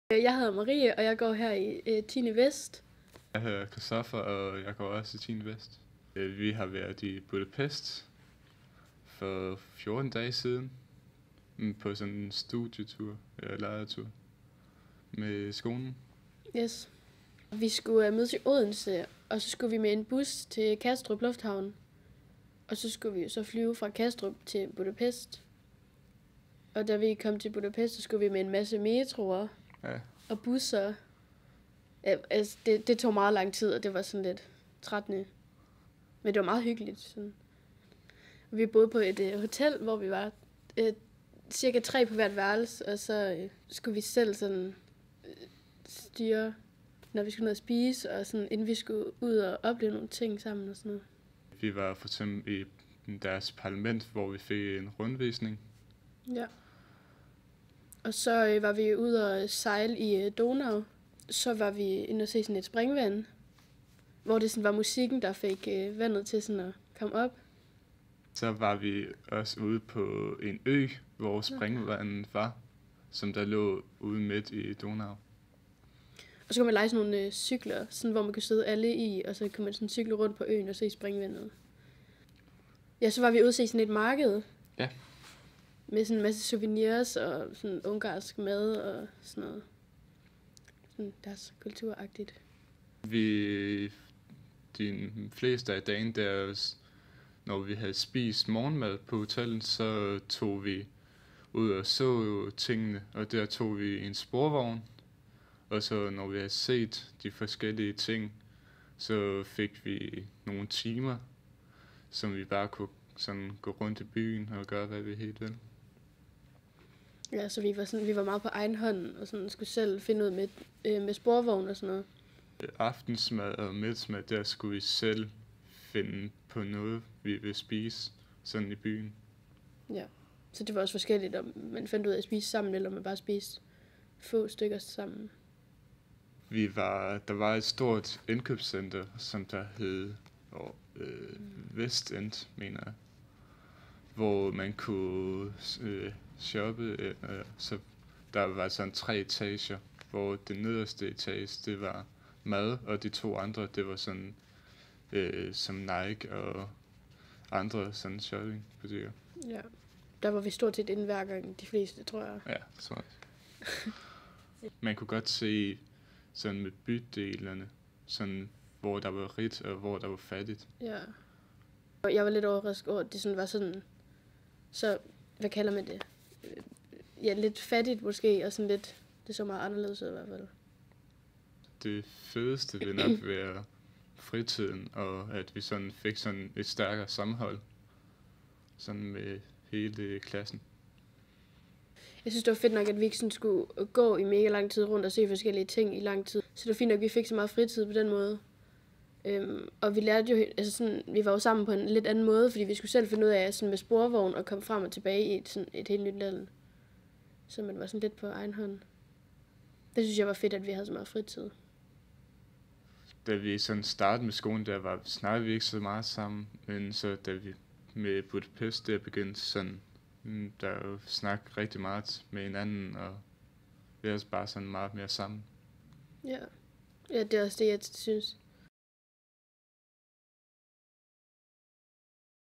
10. klasse i Budapest 2018
Hør Christoffer og Marie fra 10. Vest på Nørre Aaby Realskole fortælle om klassens studietur til Budapest.